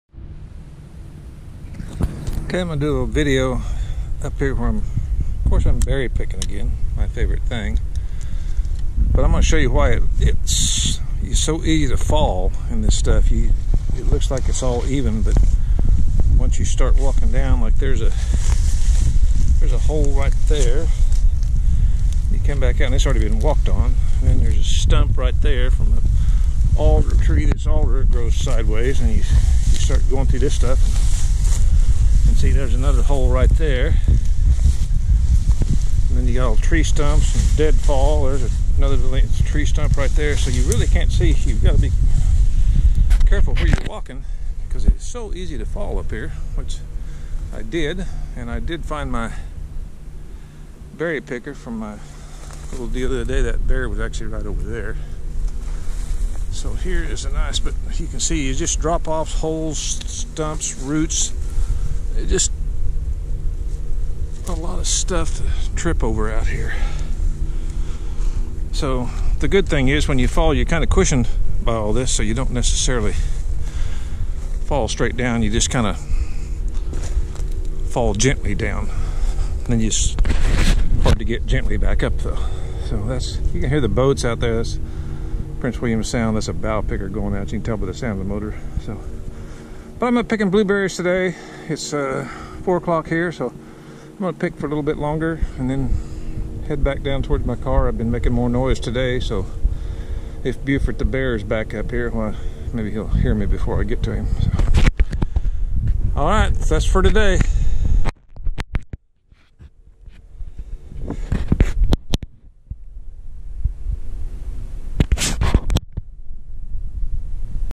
Why if difficult to walk in the woods of AK
This is why I fell the other day while backing away from the bear. The ferns and bushes cover all kinds of obstacles you step on. Sometimes its a drop off of several feet. I have learned to part the bushes before I step